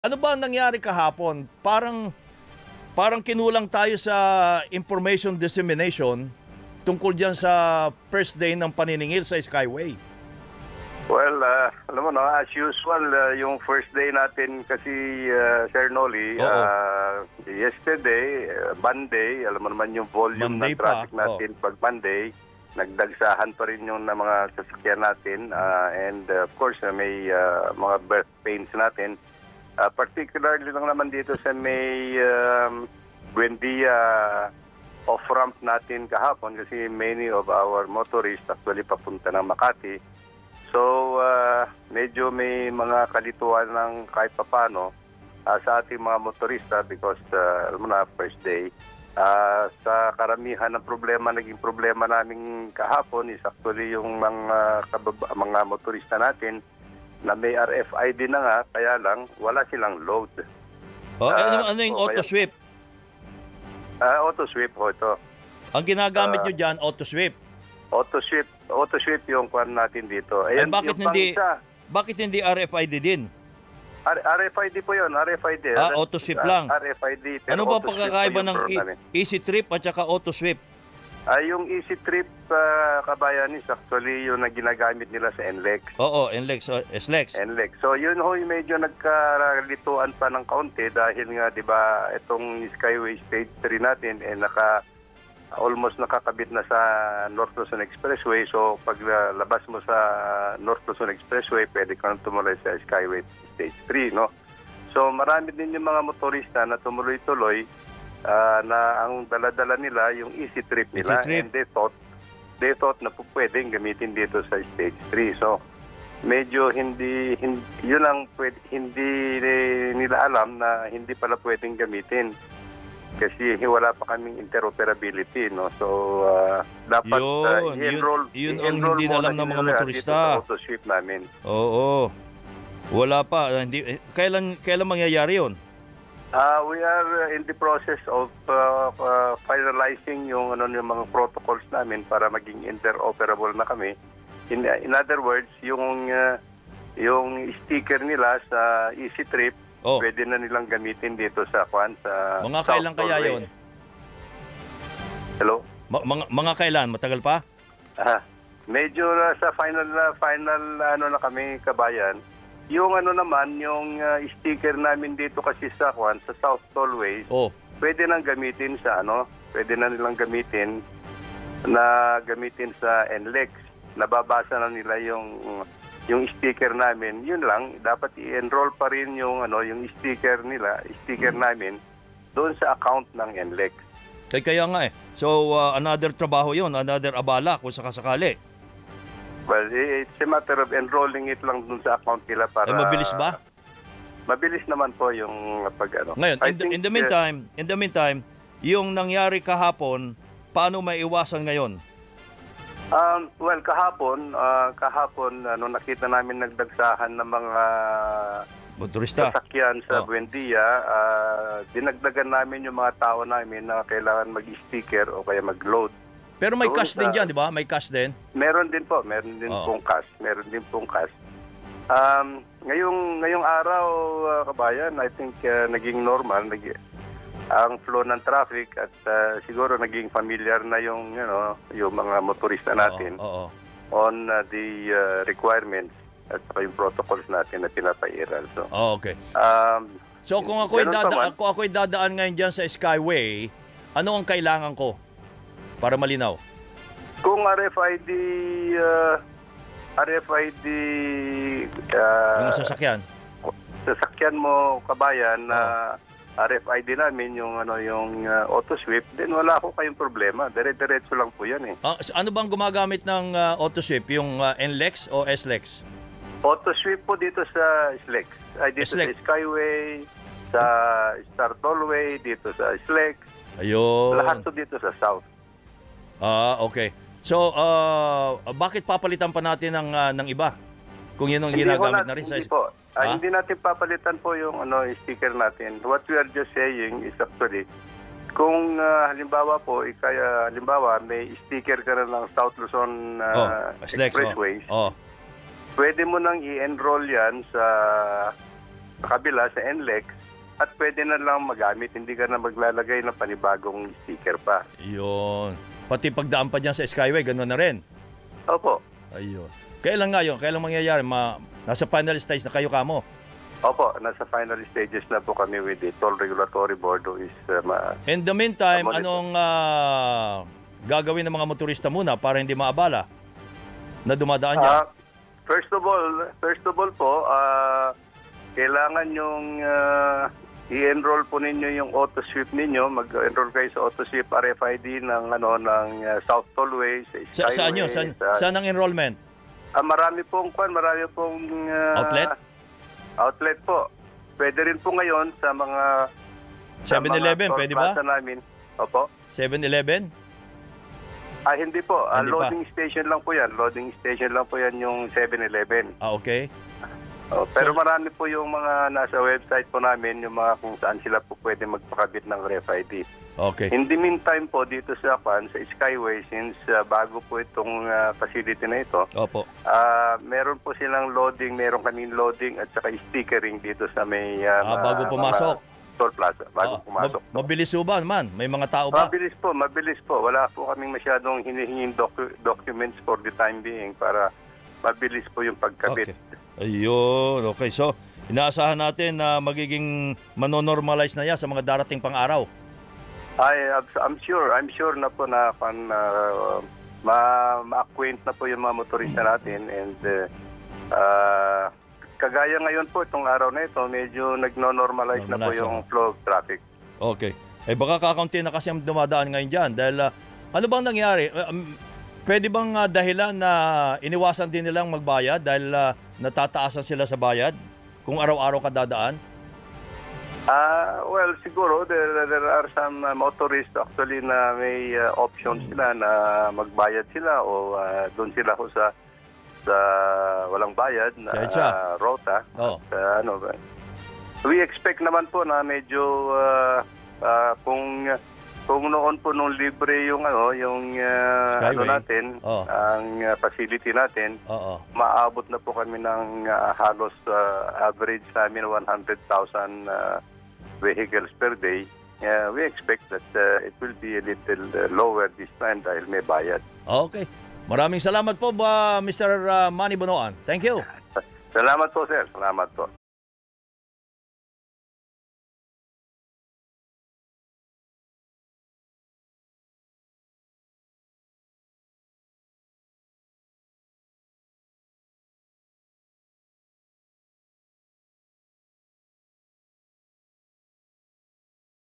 Skyway 'carmageddon' caused by confusion in payment: CEO | TeleRadyo
Confusion in the payment scheme caused a build up on Skyway Stage 3, its management said Tuesday, a day after heavy traffic was seen near toll booths of the elevated expressway.